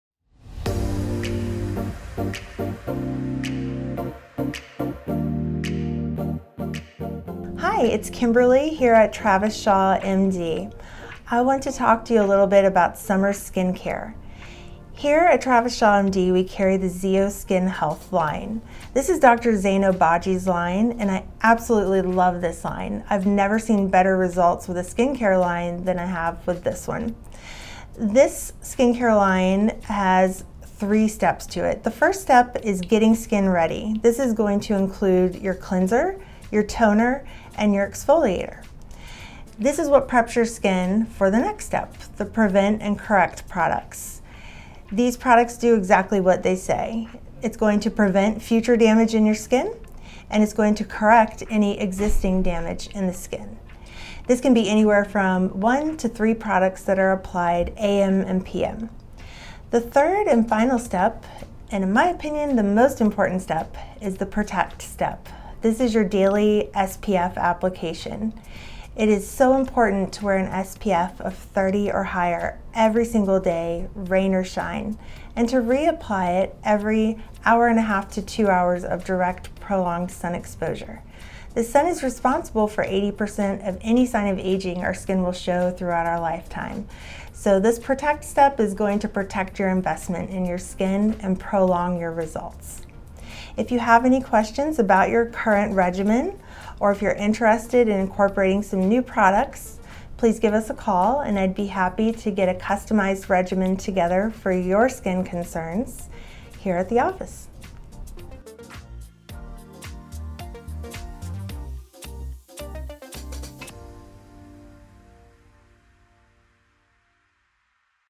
Summer Skincare - Aesthetics with Kimberly | Travis Shaw, MD.
During the summertime, our skin is at its most vulnerable. Approximately 80% of any sign of aging our skin will should throughout our lifetime, so establishing a consistent skincare regimen is key! Our favorite products to use? Check out the lines from @zeinobagiskinhealth!

Check out our blog at TravisShawMD.com for even more skincare tips.